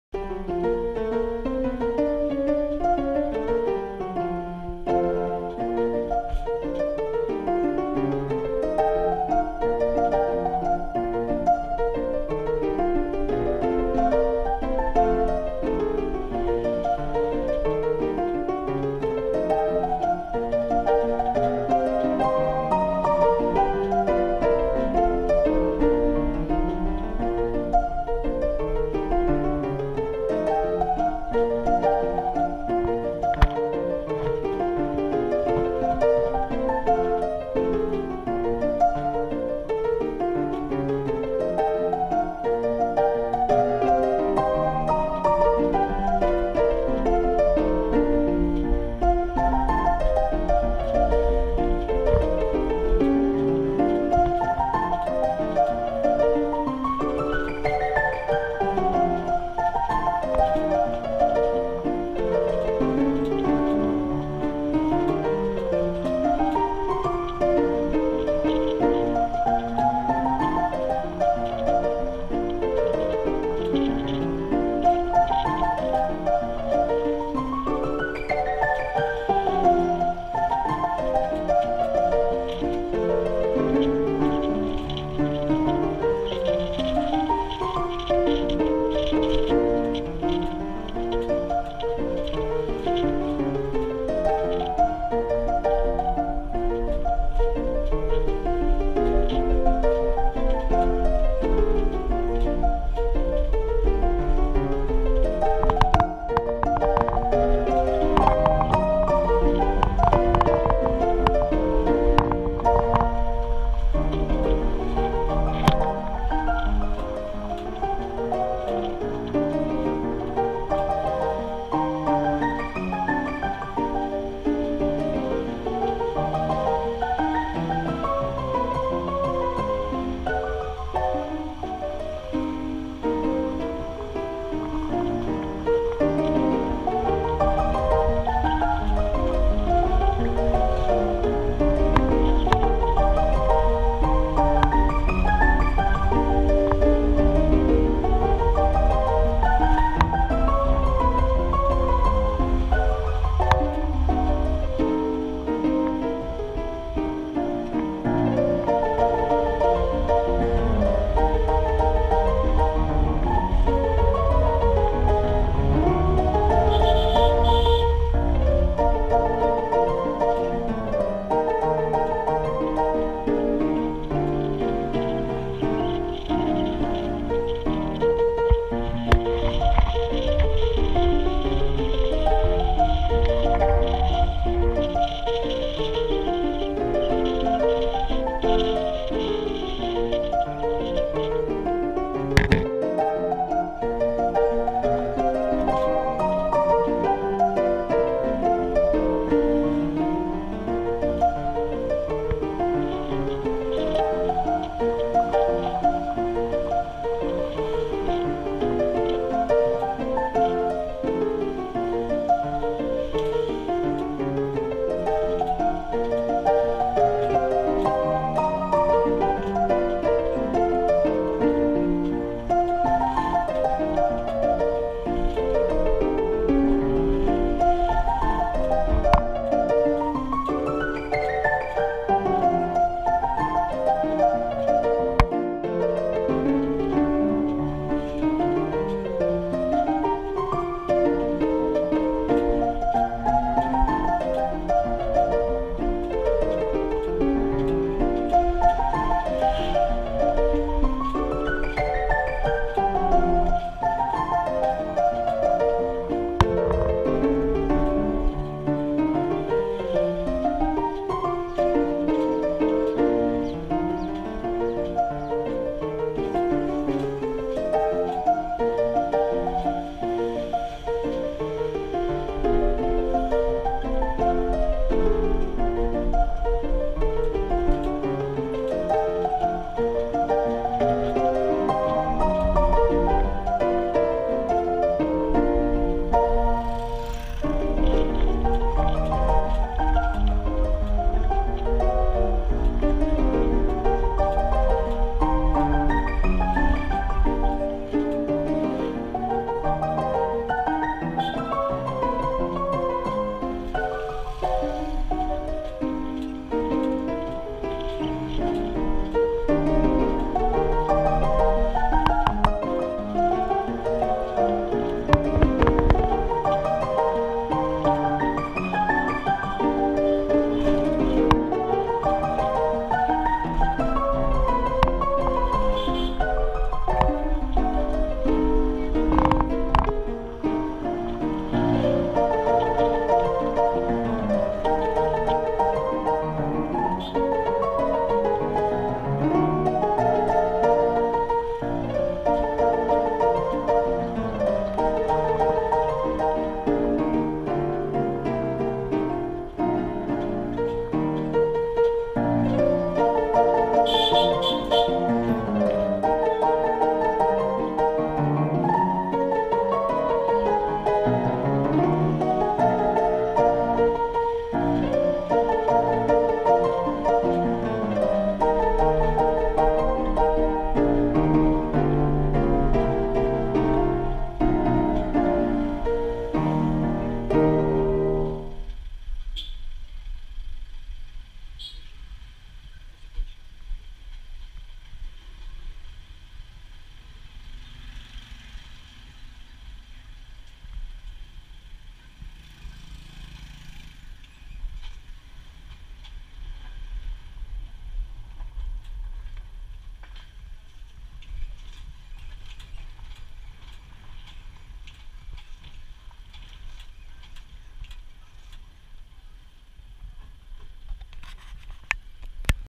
Возвращение с пляжа Samae к пирсу
На тук-туке от Samae beach до пирса за 7 минут.